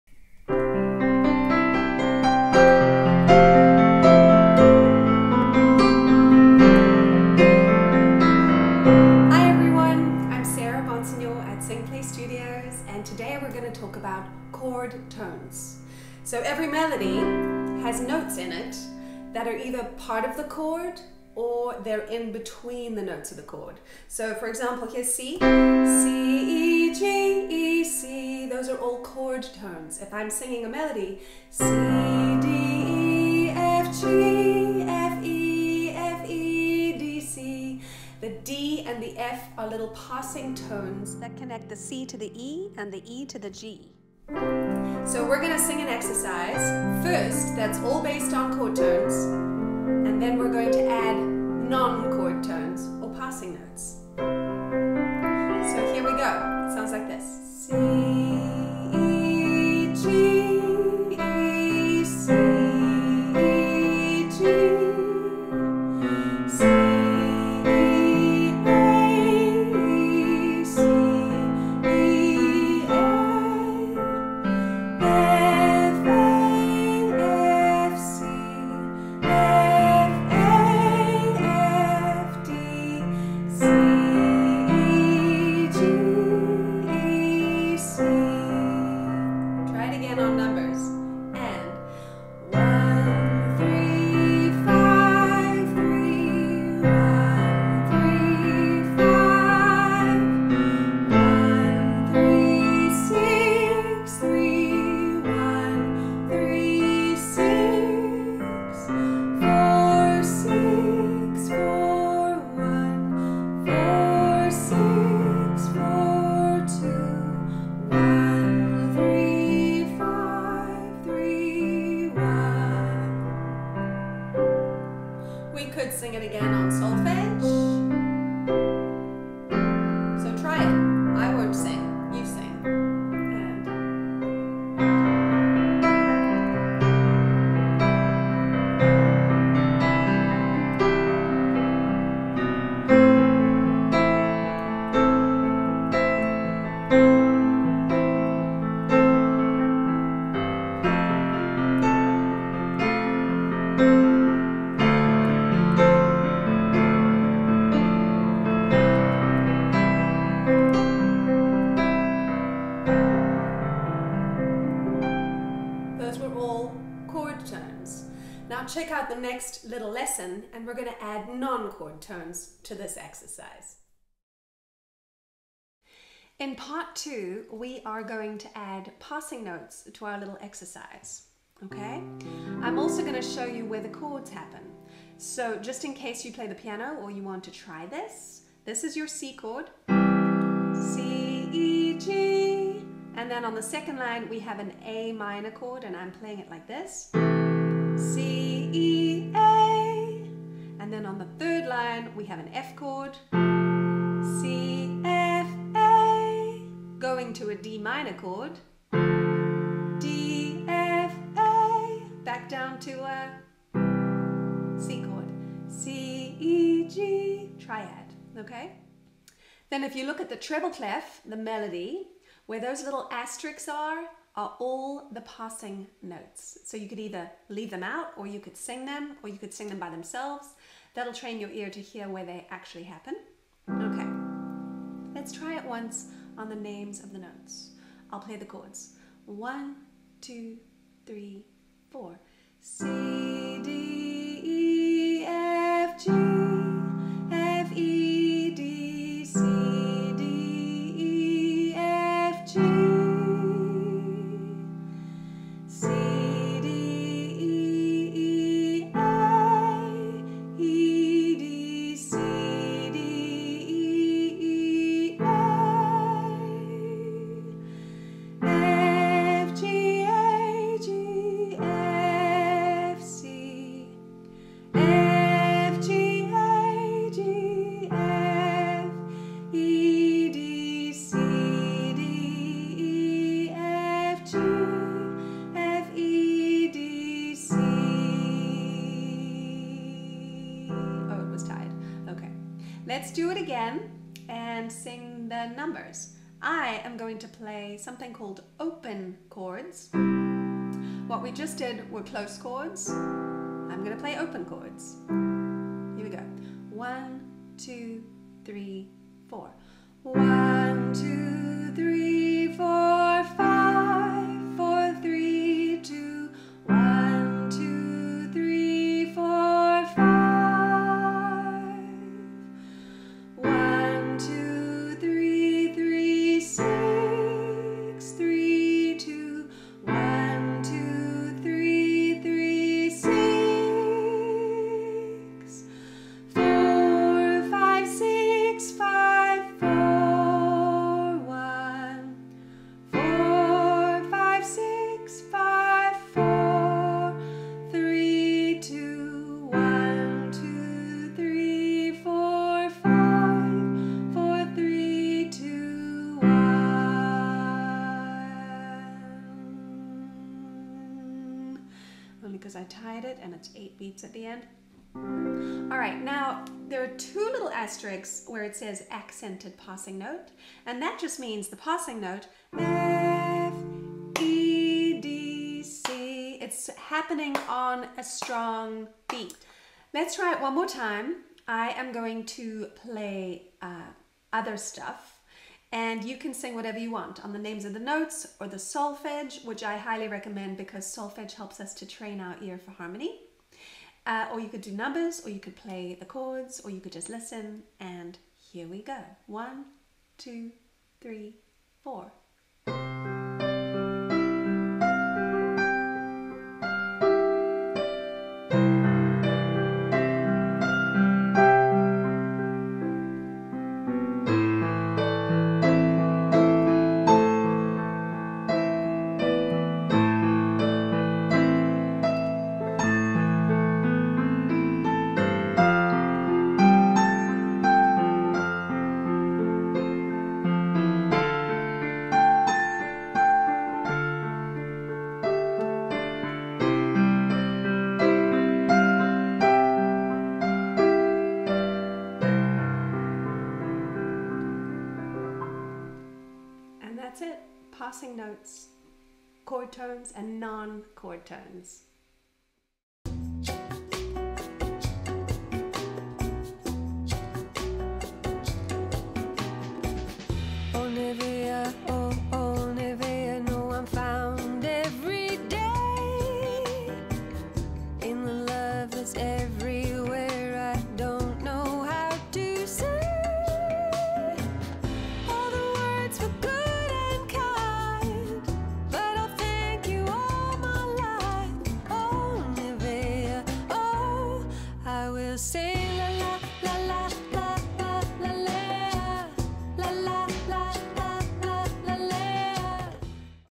Understanding Chord tones and Non-Chord tones, passing notes - part I and II
Here's a lesson in chord tones and  "passing tones" (non chord tones) by SingPlayStudios.com founder and director, Sarah Bonsignore. Practice hearing and singing passing notes, playing chords on the piano, and sight reading! Sarah also has a teaching series on Udemy.com in piano, singing and ear training for all :) Find discount links at sarahbonsignore.org

chord progression
broken chords